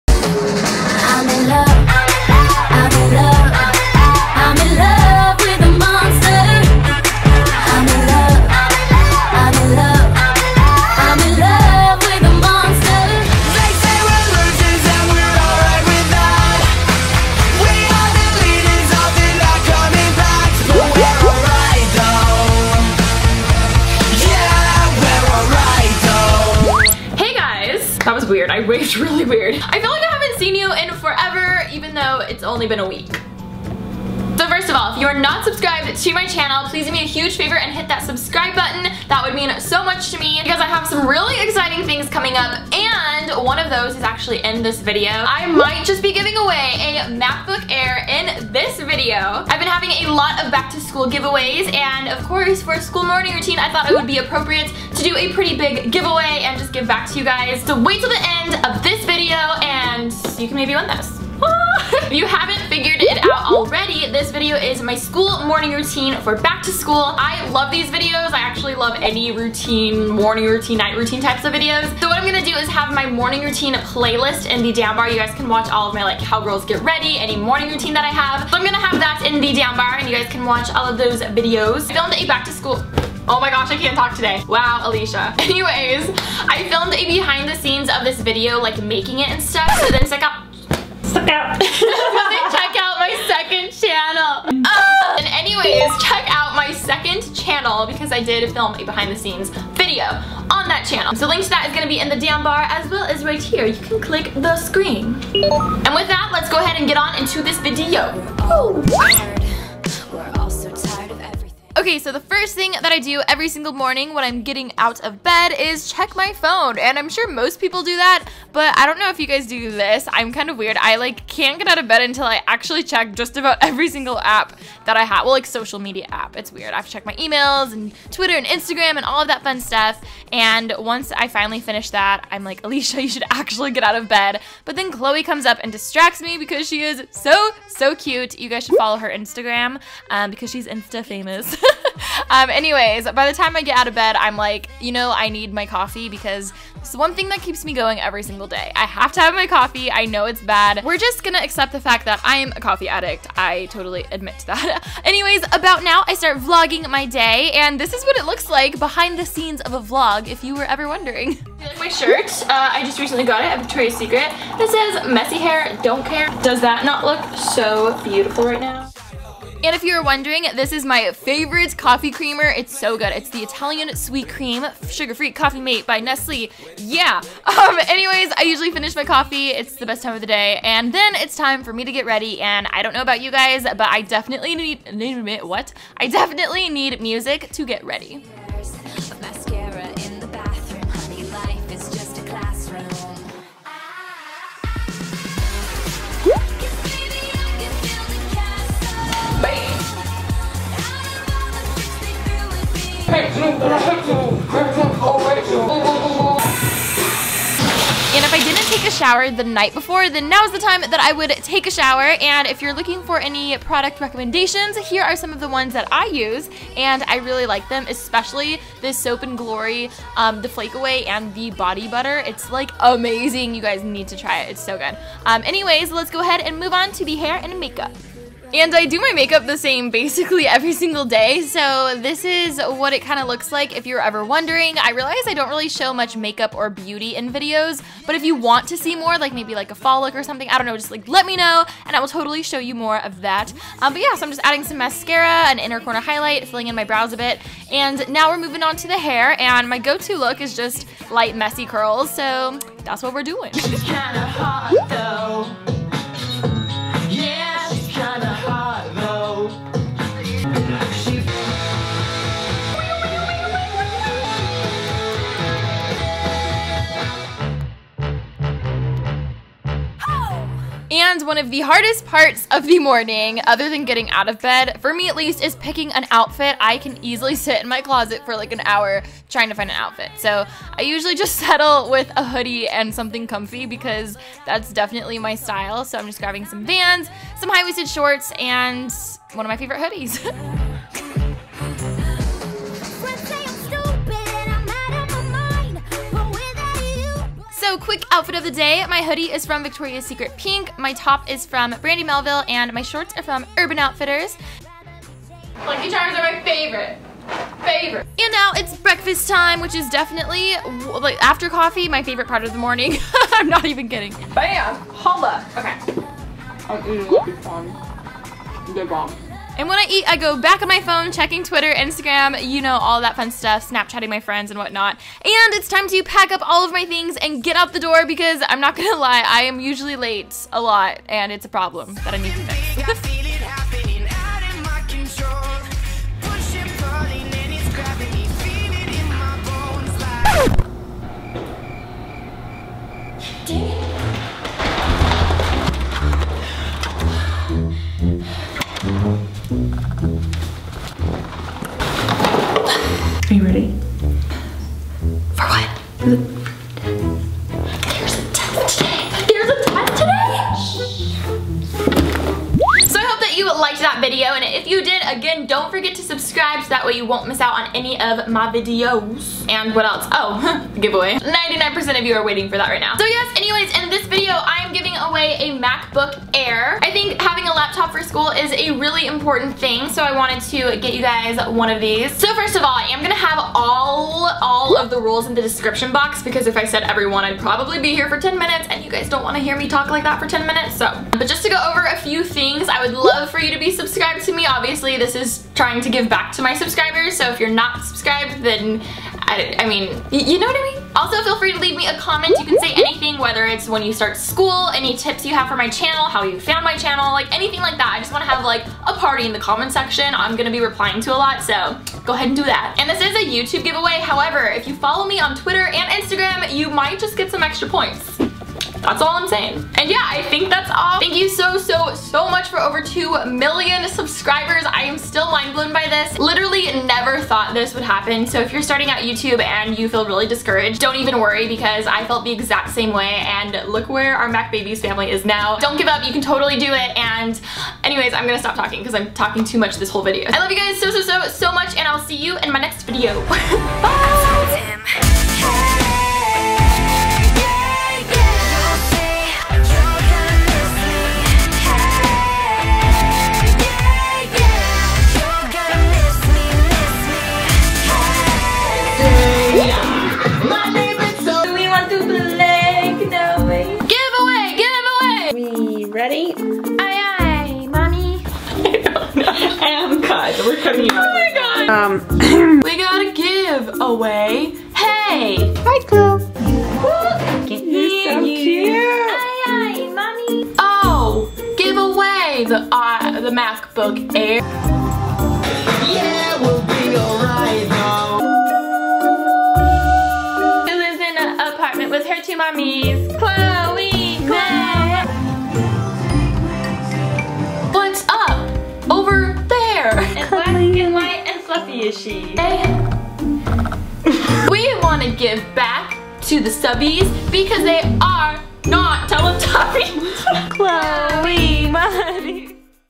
Morning Routine for School 2015!! Alisha Marie
Here is my morning routine for school! It was so requested to do new school morning routing! Love you all so much! xo -Alisha Marie

Instagram: @macbby11 & Twitter: @AlishaMarie

**IF YOU'RE READING THIS** comment" Bun on FLEEK!!"

-ends Sept 12th
-open International
-if you'd like leave me a comment saying how you found my channel or what you want to see more of!